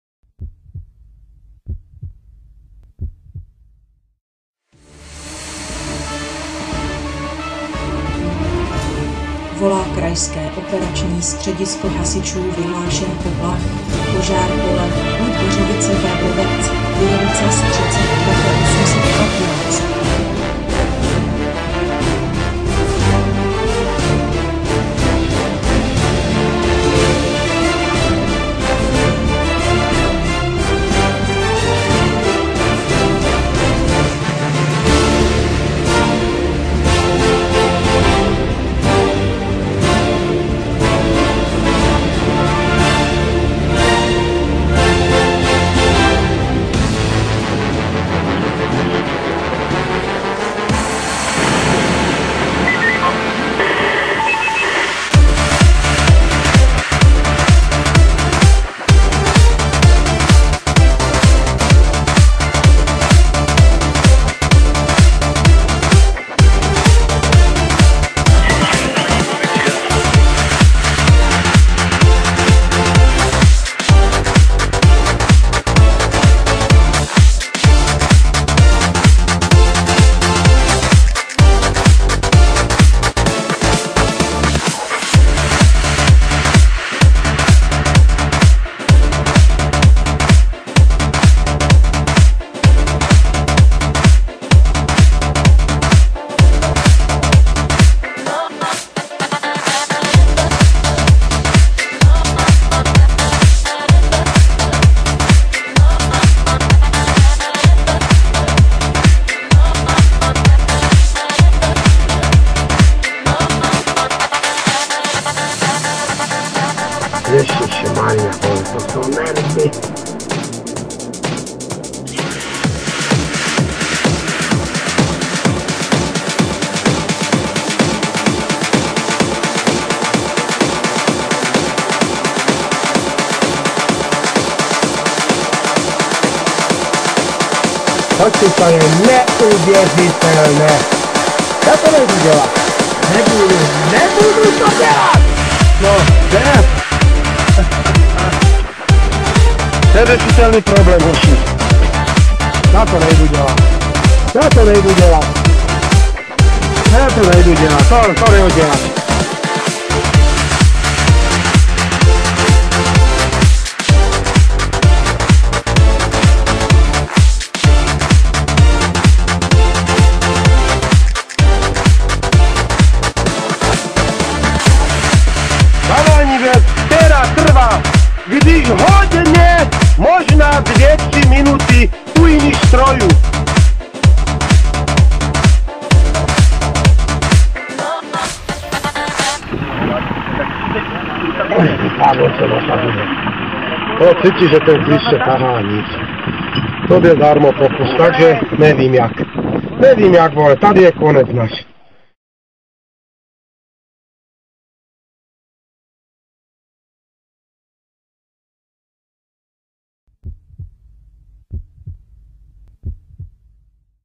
Hasiči Ludgeřovice - Požár stohu
Sestřih z výjezdu naší jednotky na požár stohu dne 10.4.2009